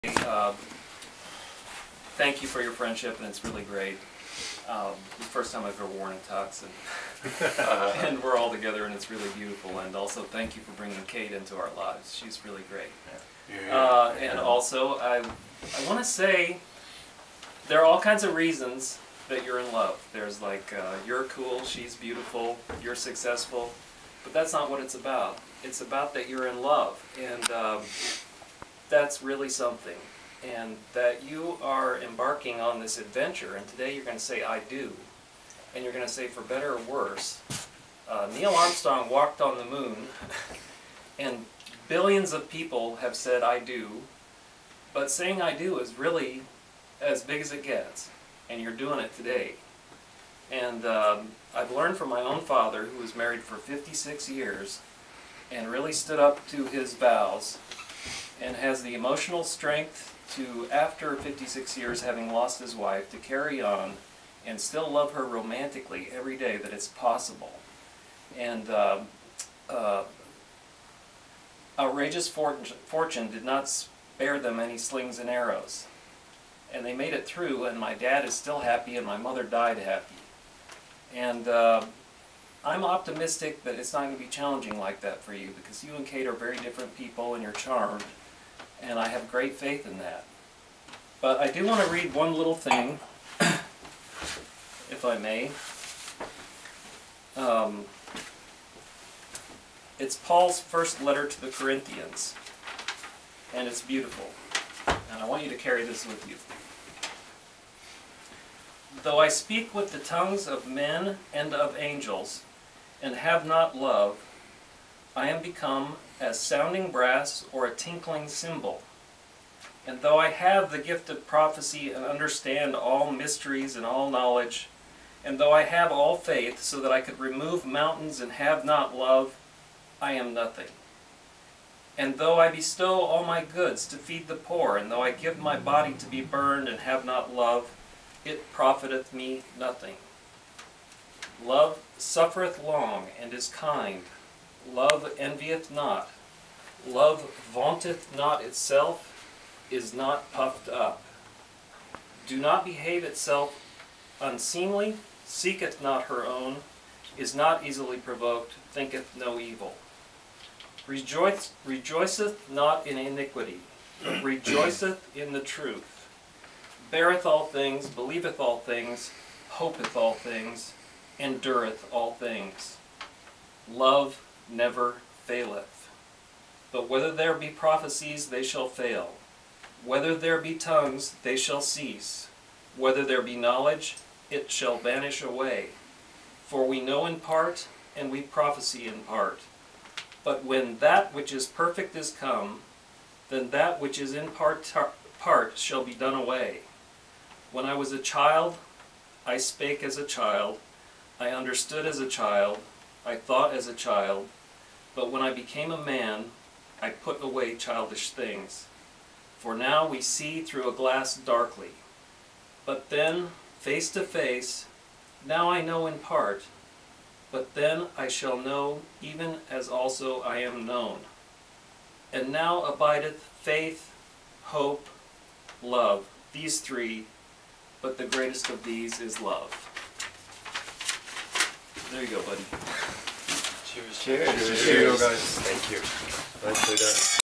Philip Heying with the groomsmen at the wedding of Jake and Cate Mills